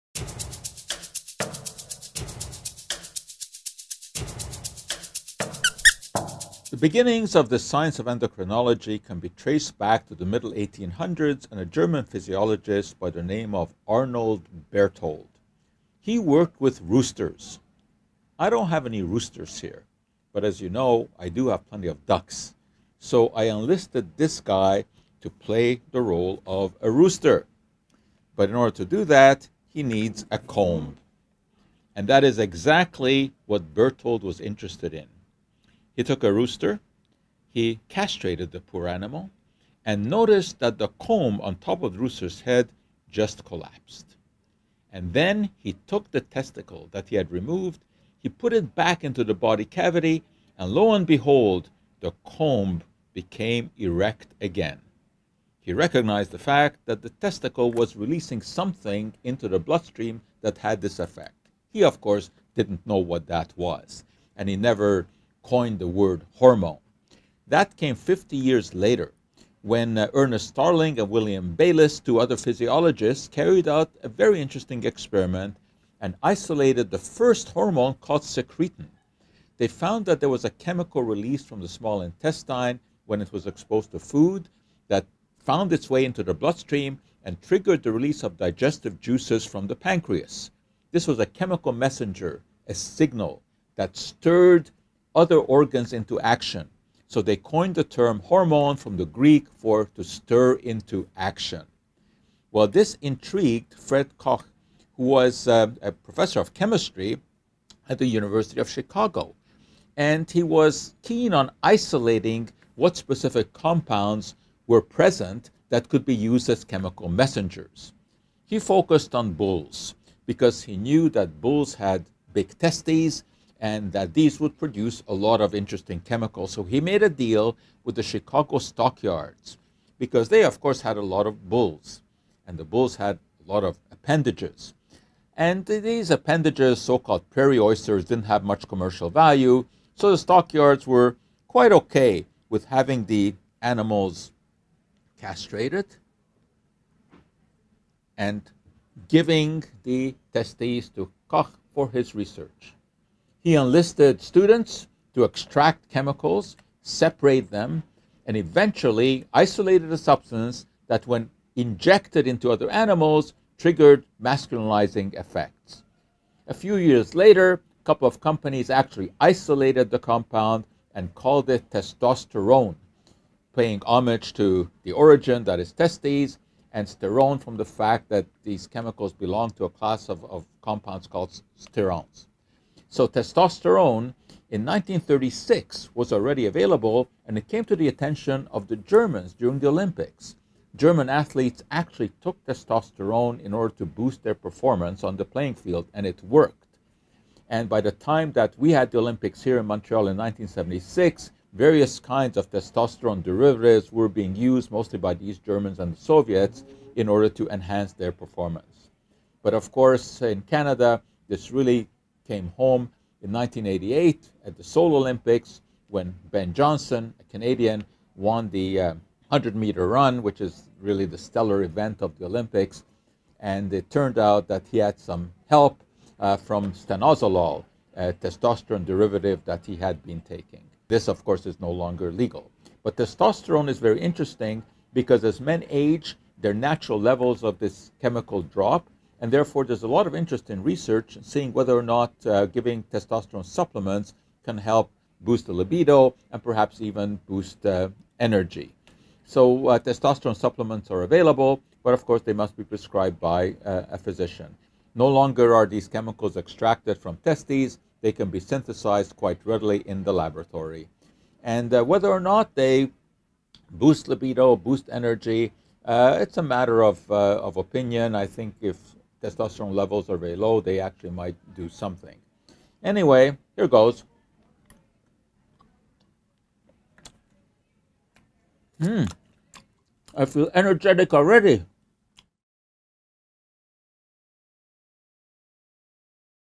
Dr. Joe Schwarcz: The discovery of testosterone
Dr. Joe Schwarcz, Director of McGill University’s Office for Science and Society, revisits the discovery of testosterone. He considers its use by athletes looking to get an edge and men looking to retain their energy as they age.